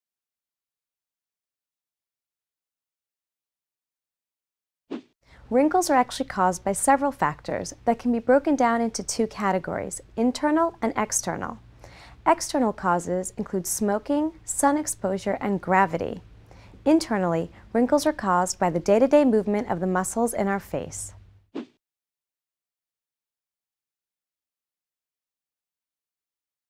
What Causes Aging Skin, Fine Lines & Wrinkles?
Dermatologist By Your Side presented by La-Roche Posay skin care experts. Our skincare professionals discuss and answer your questions on how to treat your common skin concerns!

Watch our videos to discover the causes of aging skin. Understand the internal and external factors that cause wrinkles, fine lines, smile lines, and crow's feet. Learn how to prevent early signs of aging.

Get a healthy, beautiful, younger looking complexion. Try our anti-aging skincare

For more information & online skin advice, visit our or check us out on Facebook